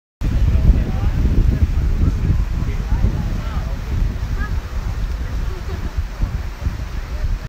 Australian water dragon
Australian vacation
the water dragon wanted to fight